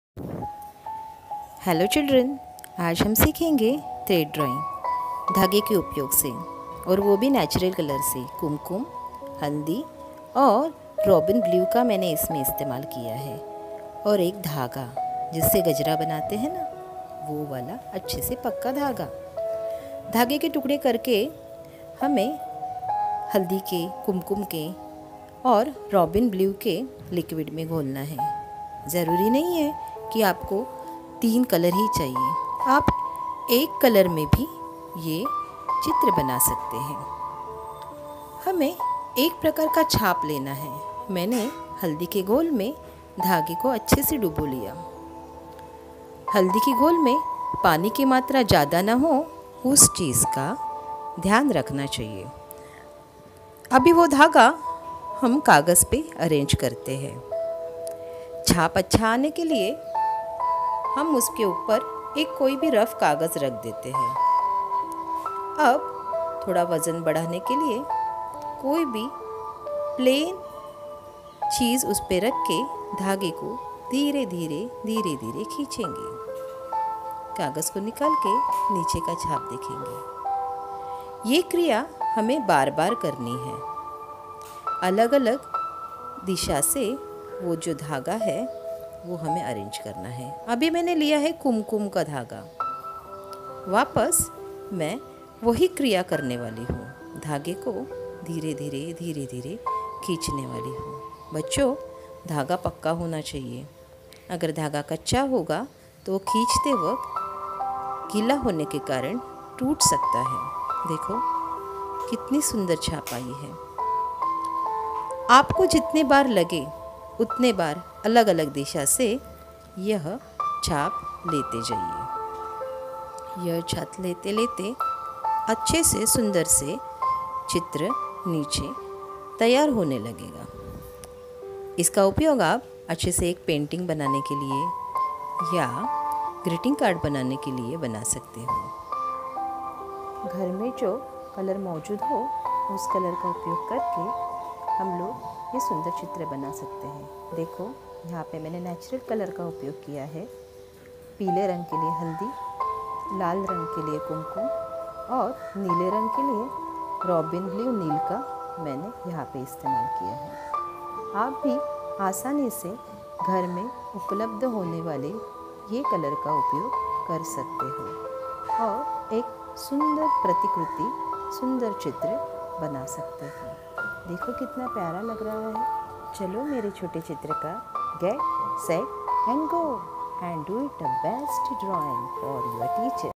Thread painting by using Natural Home made colour
Here i used Turmeric Robbin blue liquid and kumkum natural colour and thread